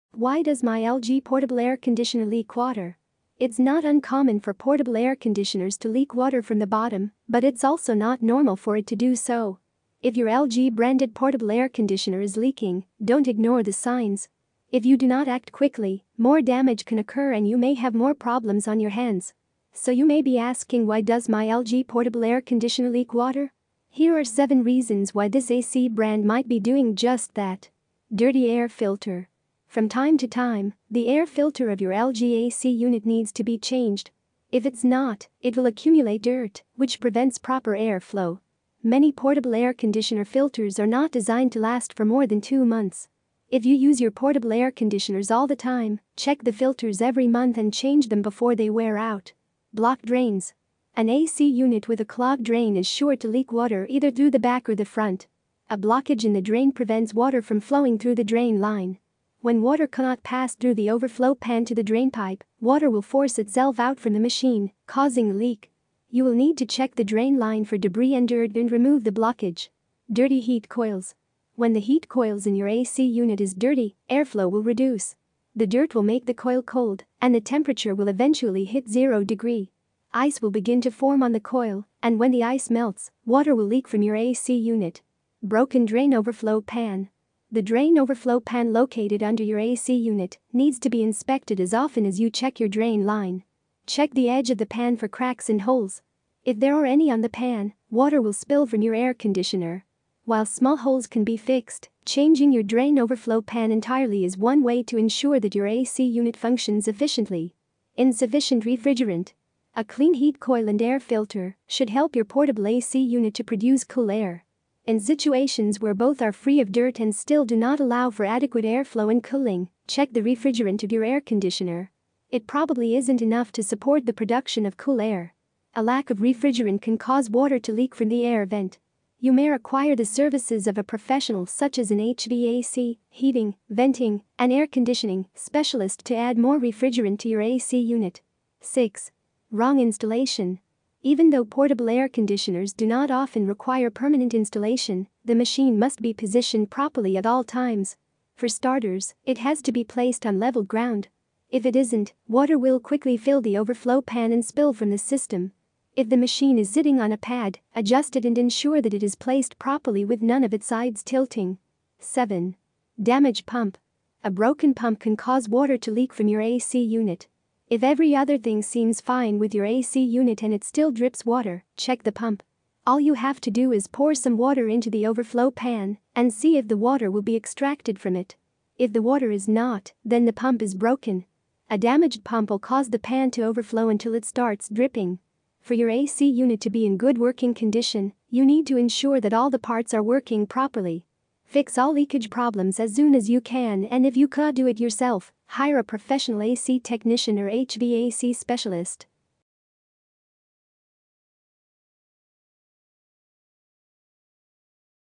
Why Does My LG Portable Air Conditioner Leak Water
It’s not uncommon for portable air conditioners to leak water from the bottom — but it’s also not normal for it to do so. If your LG-branded portable air conditioner is leaking, don’t ignore the signs. If you do not act quickly, more damage can occur and you may have more problems on your hands. So you may be asking “Why does my LG portable air conditioner leak water?” Here are seven reasons why this AC brand might be doing just that.